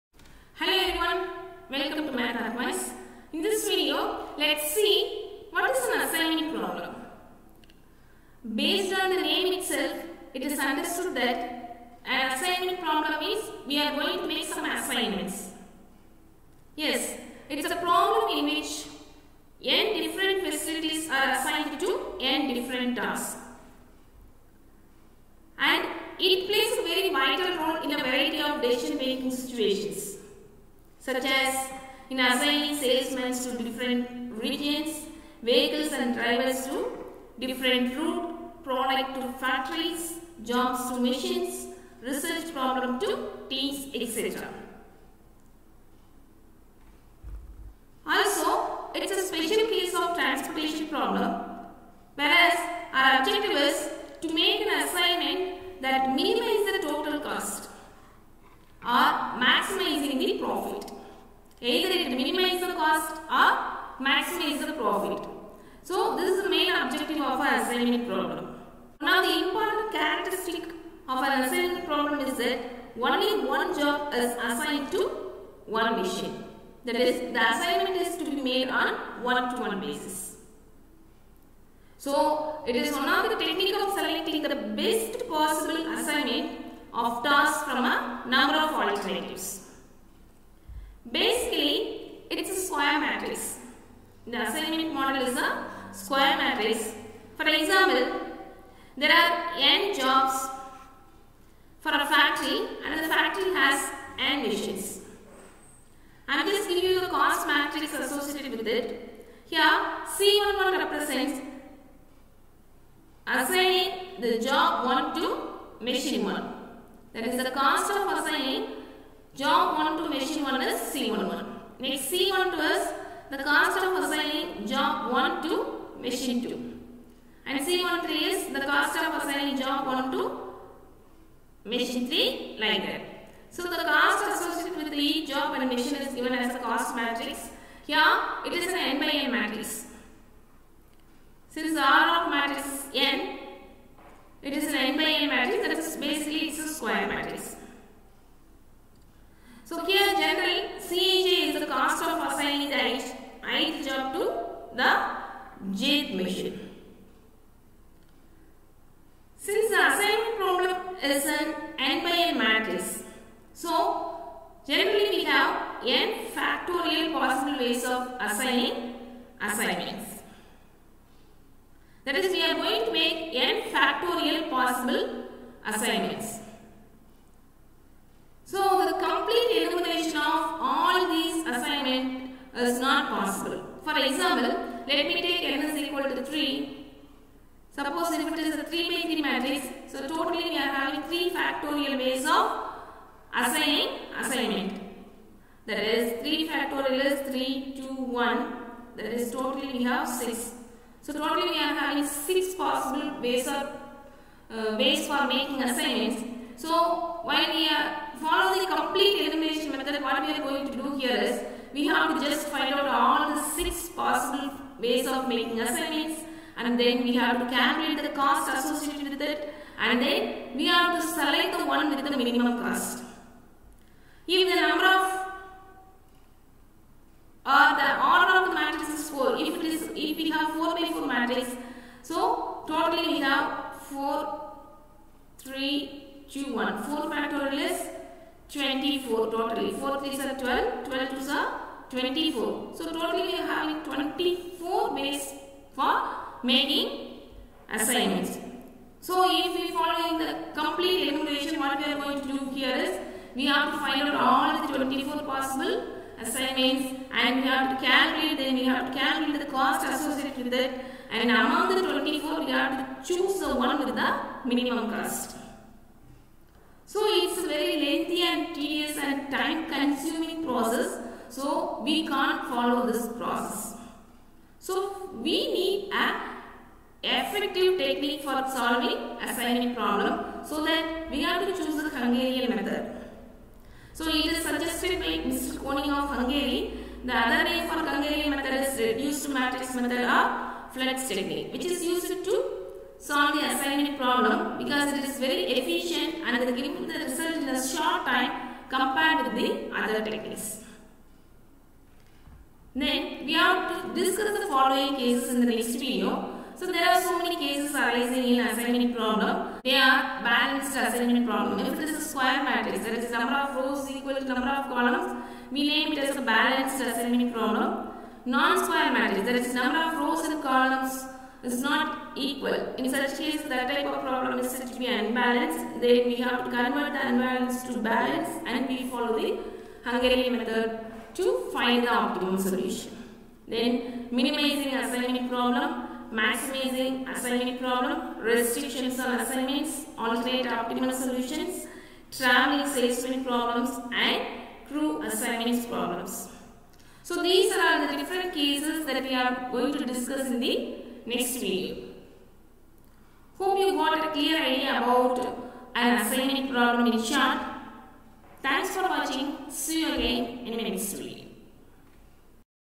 Assignment Problem - Introduction
This video is about to give an introduction of Assignment Problem in short.